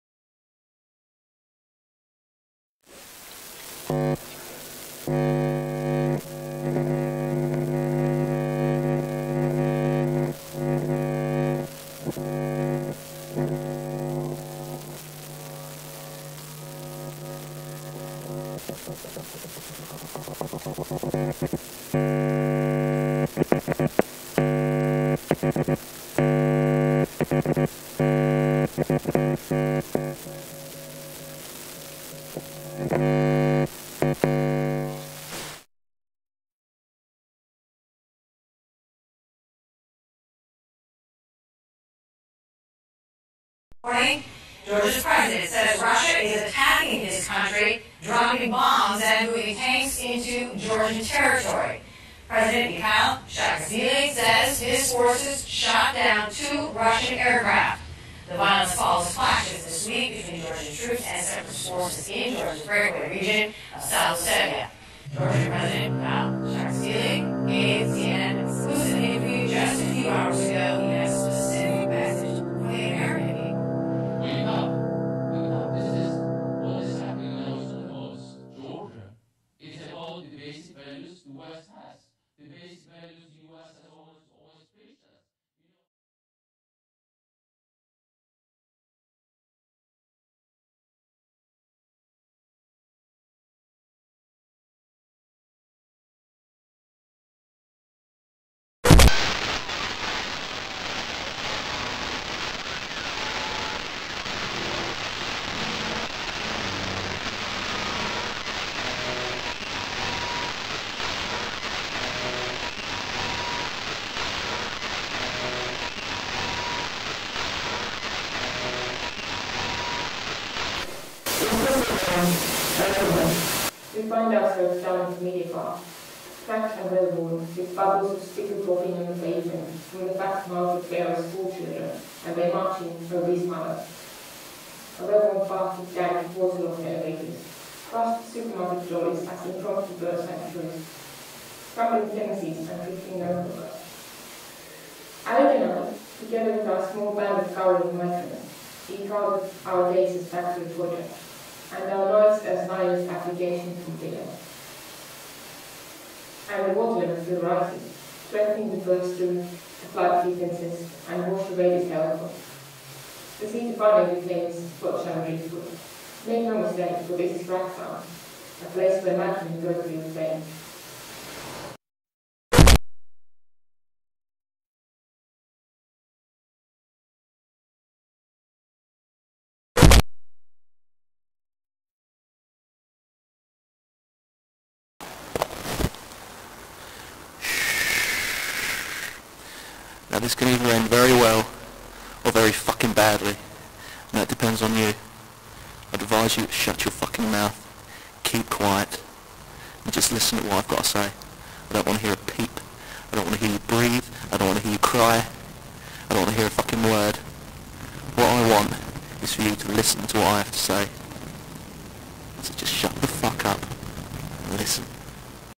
Cracktown v2.0; Bitter Stimulus [1/5; Grey Slate]
We find ourselves down a familiar path -- cracked and well-worn, with bubbles of spittle popping on the pavement from the fat mouths of feral schoolchildren and their marching, obese Mothers; a well-worn path with dank, water-logged elevators, rusted supermarket trollies as impromptu bird sanctuaries, crumbling tenancies and drifting no-hopers.

Alik and I, together with our small band of cowering migrants, eke out our days as factory fodder, and our nights as nihilist application form fillers. And the water level still rises, threatening to burst through the flood defenses and wash away this hellhole -- the sea to finally reclaim its flotsam and driftwood. Make no mistake - for this is Cracktown; A place where madmen go to feel sane.